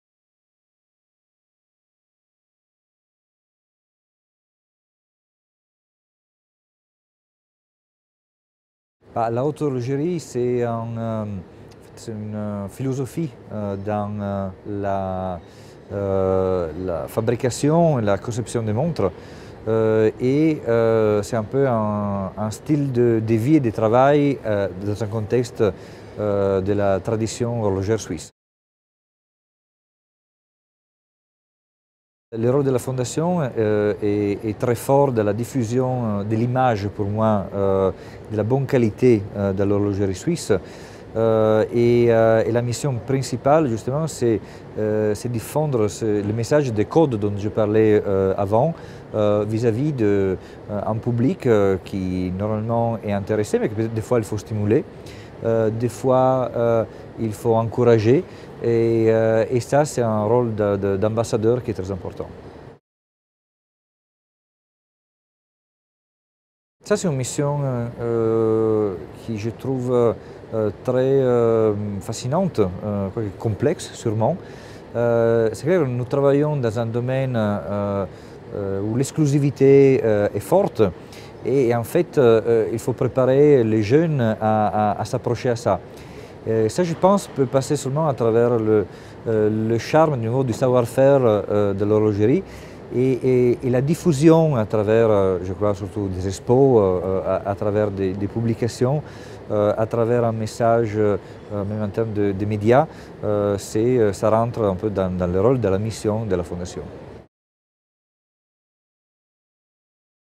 Interview with Stefano Macaluso, CEO, Girard-Perregaux
Find out what the Chief Executives of the Fondation de la Haute Horlogerie partner-brands have to say in interviews every Monday and Thursday on HH Magazine.

In less than three minutes, they share their views on the following questions:

• What's your definition of Fine Watchmaking?
• As a partner to the FHH, what would you say are the Foundation's priorities?
• What can be done to interest younger generations in Fine Watchmaking?

When it comes to Fine Watchmaking and safeguarding this unique heritage,
rest assured they have plenty to say.